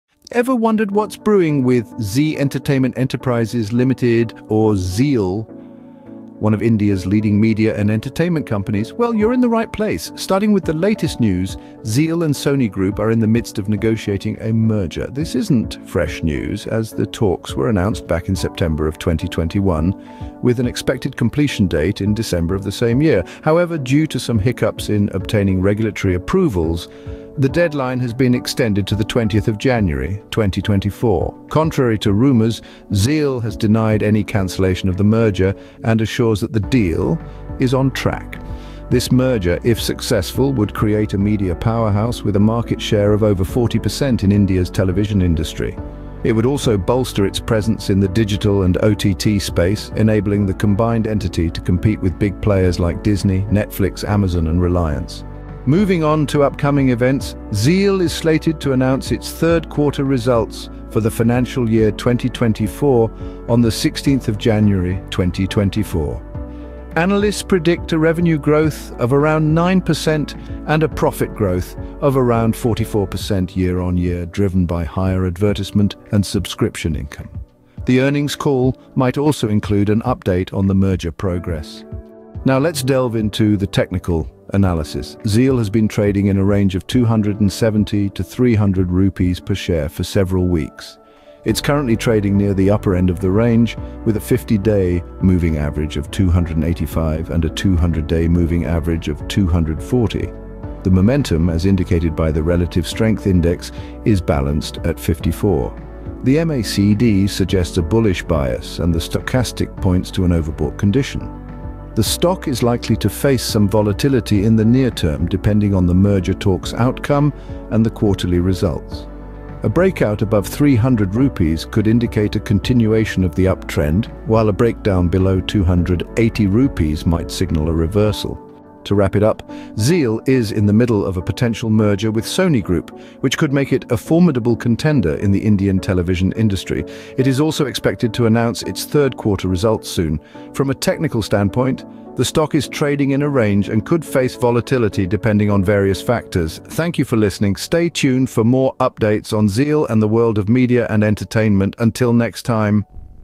Zee Entertainment Enterprises Limited Latest News and Analysis | Fundcode
ZEEL: The Media Giant in Making by fundcode's Workspace

OUTLINE:

00:00:00 ZEEL-Sony Merger Negotiations
00:00:55 ZEEL's Third Quarter Results Announcement
00:01:22 Technical Analysis of ZEEL Stock
00:02:08 Conclusion